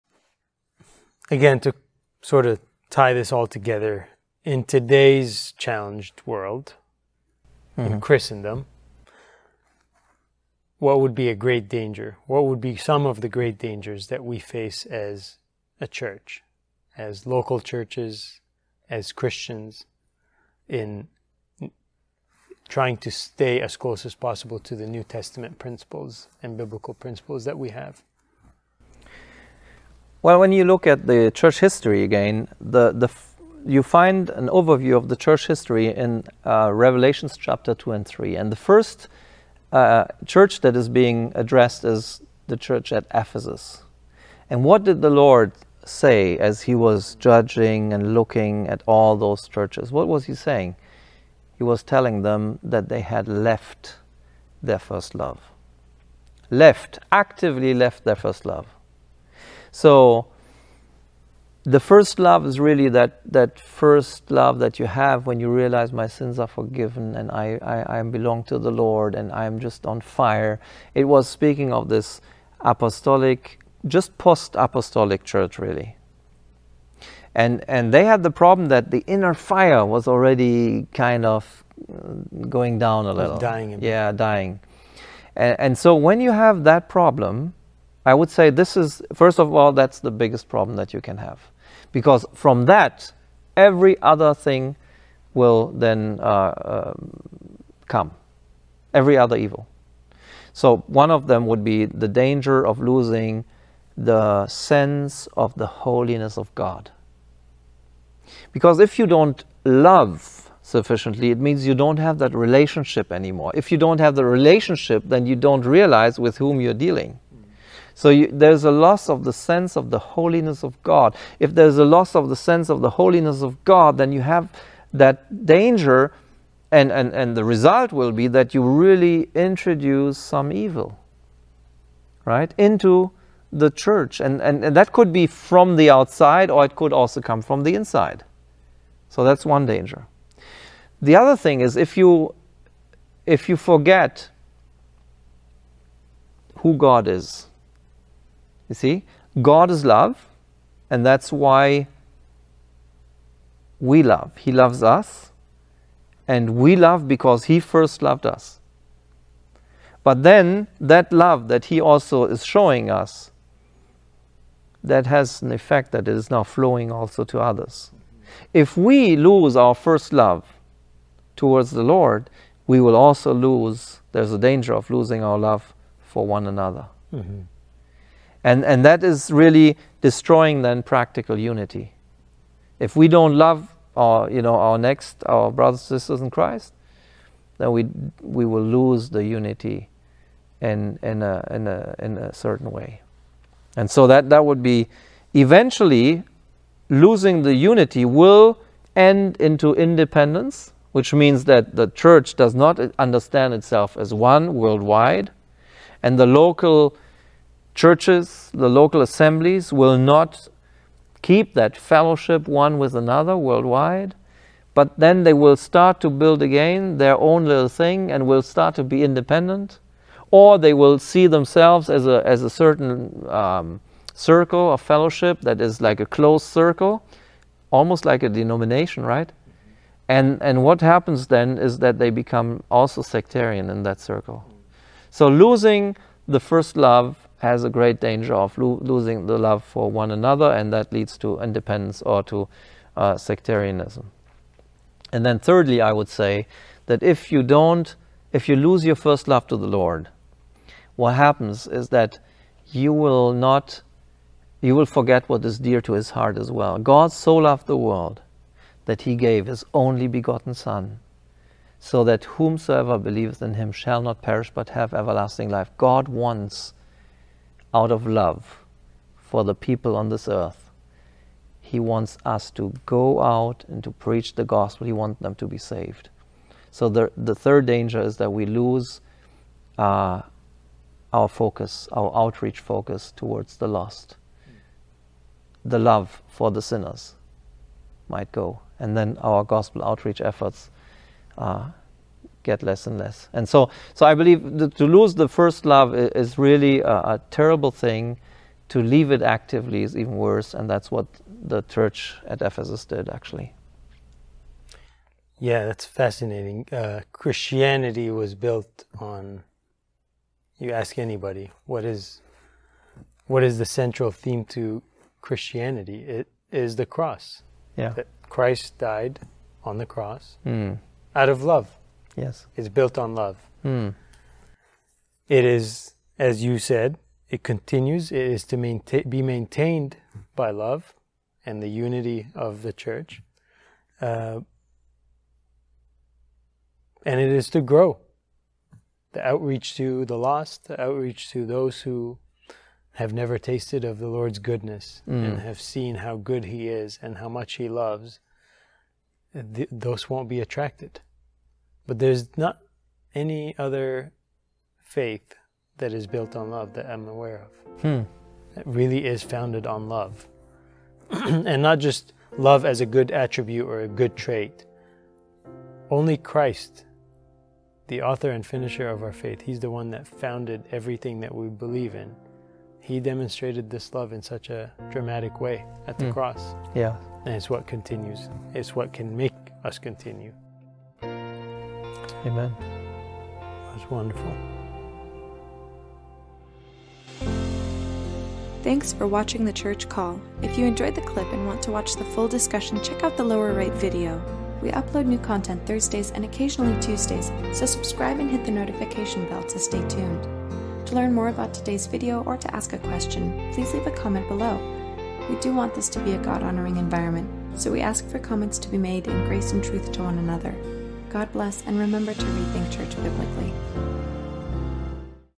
What're the greatest dangers for the local church today?
The local church is under assault, and Satan is trying to destroy it. But the gates of Hades shall not prevail! Christ is building His Church, and He wants to use us to do it. However, to build well, we need to be aware of the imminent dangers. Join Daniel and Joe in the final segment of their discussion as they analyze what the greatest dangers are for the local church today!